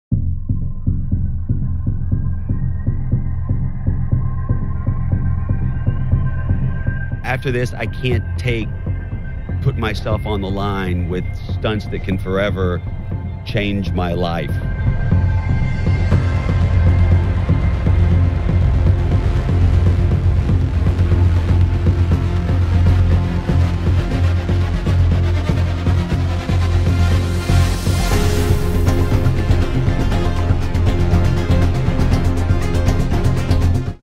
Jackass Forever Puts More Animals Than Humans at Risk
Sure, @JohnnyKnoxville can choose to retire from dangerous stunts, but the animals he torments in JackassForever don’t get that choice.

The film puts more animals at risk than humans — don’t watch it!